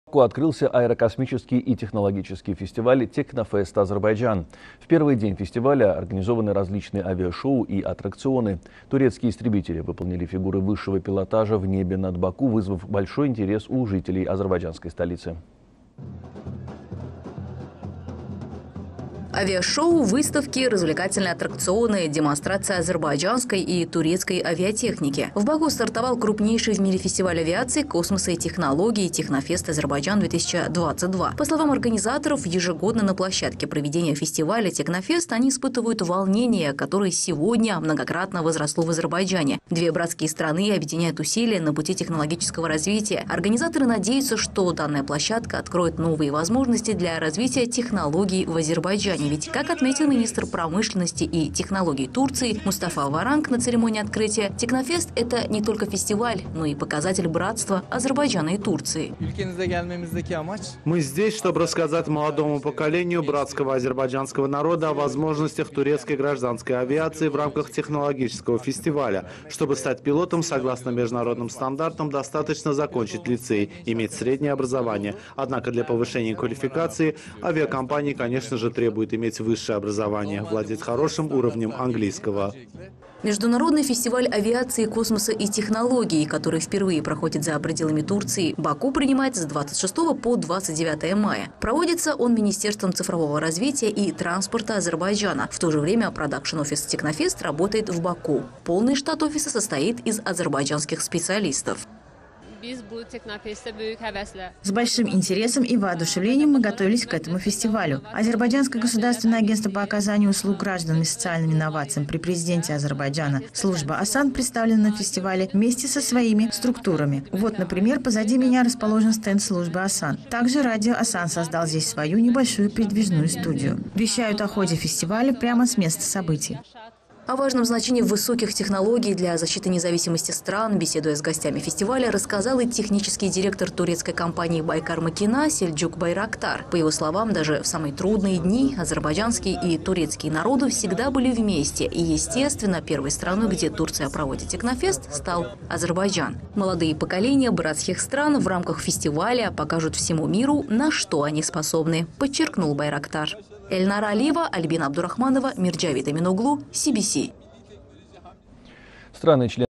“ASAN xidmət” "Teknofest – Azərbaycan"da - CBC Azərbaycan 26.05.2022
Bakıda “Teknofest  - Azərbaycan” -  aviasiya, kosmos və texnologiya festivalına start verilib.

29 may tarixinədək davam edəcək festivalın təşkilatçıları “Türkiyə Texnologiya Komandası” - T3 Fondu, Azərbaycan Respublikasının Rəqəmsal İnkişaf və Nəqliyyat Nazirliyi, Türkiyə Respublikasının Sənaye və Texnologiya Nazirliyidir.

Azərbaycan Respublikasının Prezidenti yanında Vətəndaşlara Xidmət və Sosial İnnovasiyalar üzrə Dövlət Agentliyi festivalın tərəfdaşıdır.
Ümumilikdə Festivalda təşkil olunan sərgi pavilyonlarında Dövlət Agentliyinin müxtəlif qurumlarının və struktur bölmələrinin -   “Elektron Hökumətin İnkişafı Mərkəzi”nin, “ABAD”ın, “YAŞAT” Fondunun, ASAN Radionun, “İnnovasiyalar Mərkəzi”nin innovativ məhsul və həlləri nümayiş olunur.

Həmçinin Azərbaycan Respublikasının Prezidenti yanında Vətəndaşlara Xidmət və Sosial İnnovasiyalar üzrə Dövlət Agentliyi festival çərçivəsində təşkil olunan “Sosial yönümlü texnologiyalar” müsabiqəsinin təşkilatçısıdır.
Türkiyə Texnologiya Komandası Fondunun (T3) pavilyonunda “Bilim Bakı” mərkəzinin stendi də təqdim olunur.